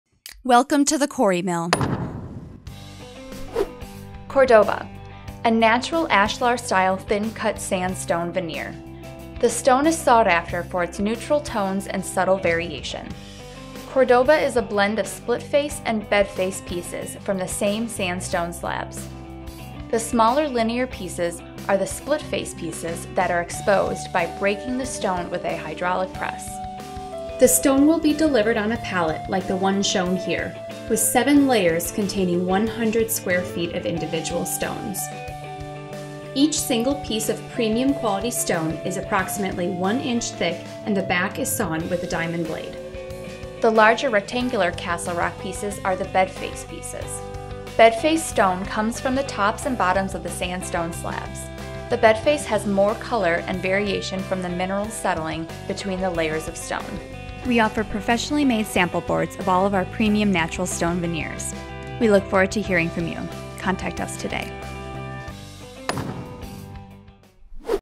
Cordoba Neutral Ashlar Style Real Thin Stone Veneer
Cordoba is a natural ashlar style thin cut sandstone veneer. The stone is sought after for its neutral tones and subtle variation. Cordoba is a blend of splitface and bedface pieces from the same sandstone slabs. The smaller linear pieces are the splitface pieces that are exposed by breaking the stone with a hydraulic press. The larger rectangular castle rock pieces are the bedface pieces. Bedface stone comes from the tops and bottoms of the sandstone slabs. The bedface has more color and variation from the minerals settling between the layers of stone. By incorporating both faces of the stone into the product, Cordoa has a cohesive blend representing the range of colors that come from the quarry.